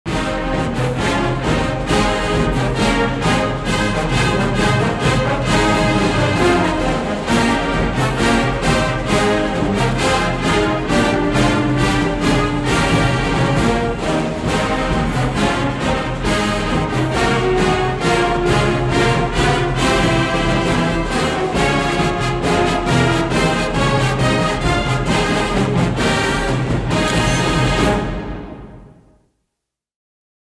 The W Project time-lapse
Hundreds of first-year University of Wisconsin–Madison students gathered for the W Project, forming a giant motion W on Barry Alvarez Field at Camp Randall Stadium.